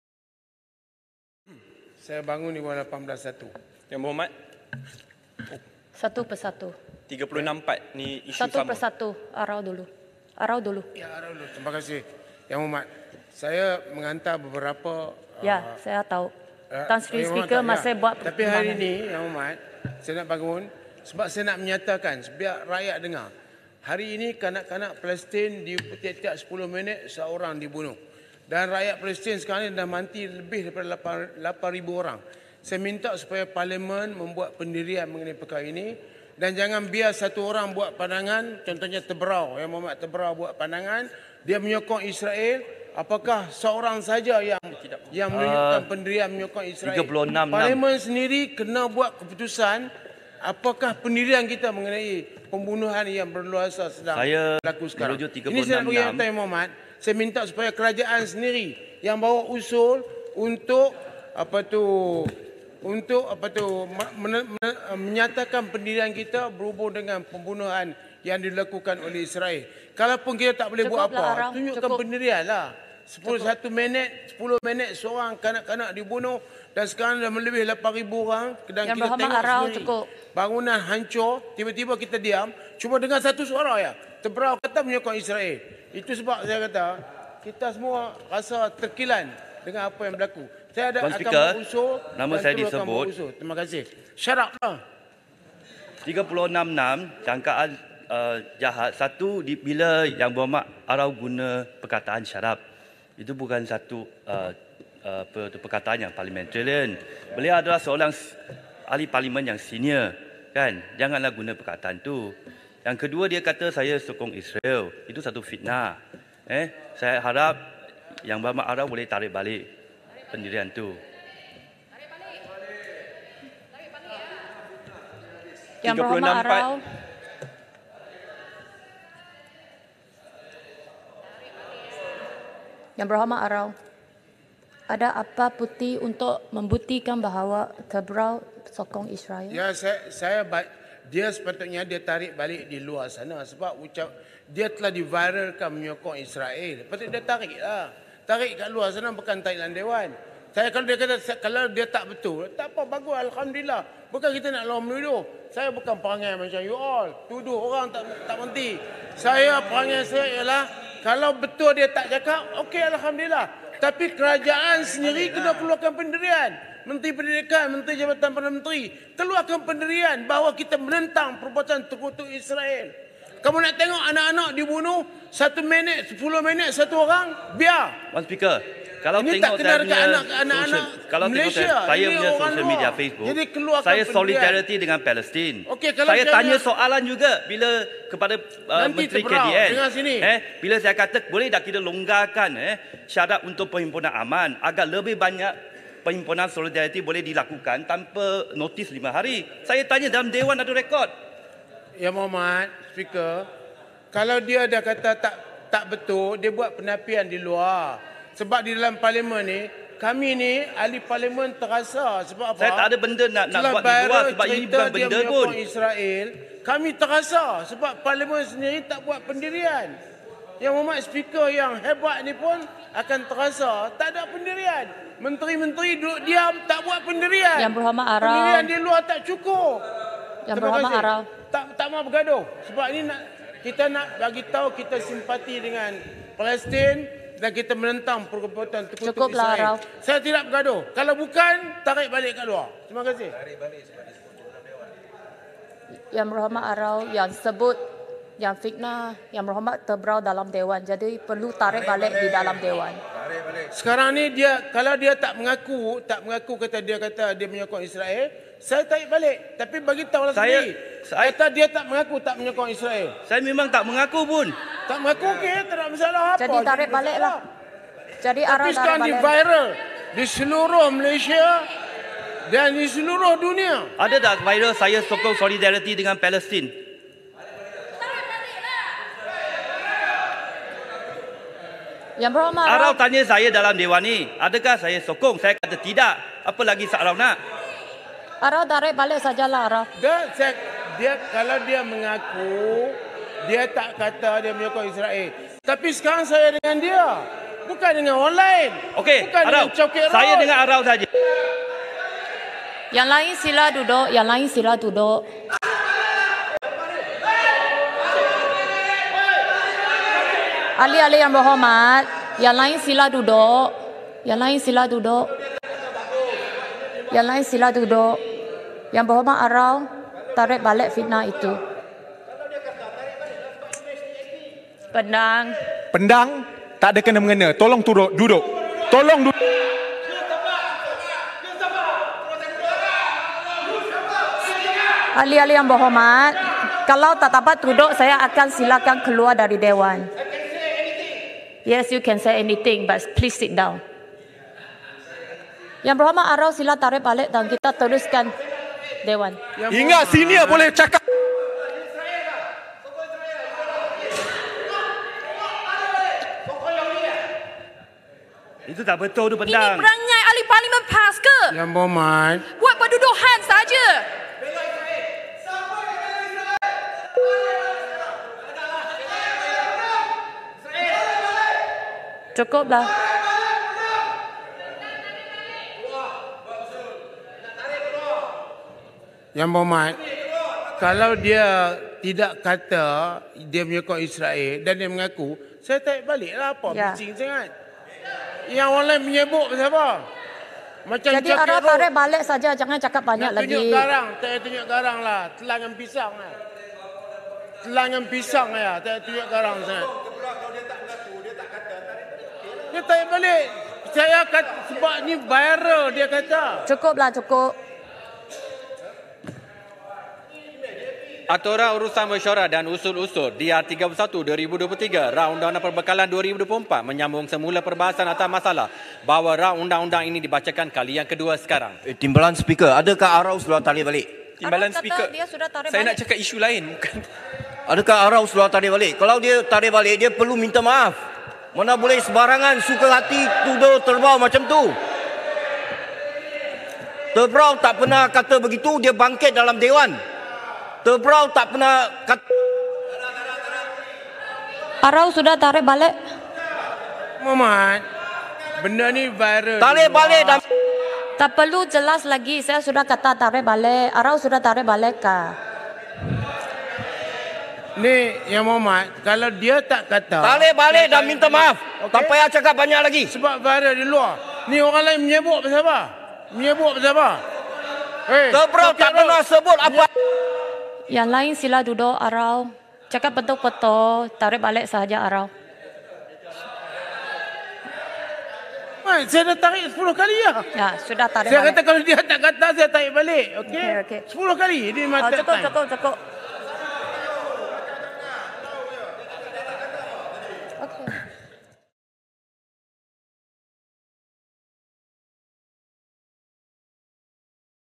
MGNews: Dewan Rakyat Kecoh Selepas Shahidan Dakwa Ahli Parlimen Tebrau Sokong Israel
Timbalan Yang Dipertua Dewan Rakyat, Alice Lau meminta Ahli Parlimen Arau, Datuk Seri Shahidan Kassim menyertakan bukti mengenai kenyataan yang dibuat.